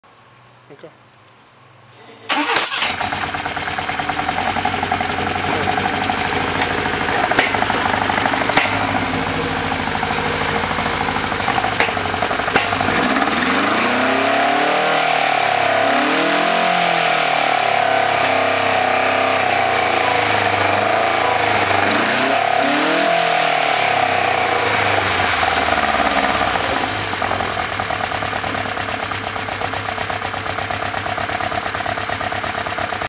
1976 BMW First Start Second Car
This time around it had an exhaust on it, not open header.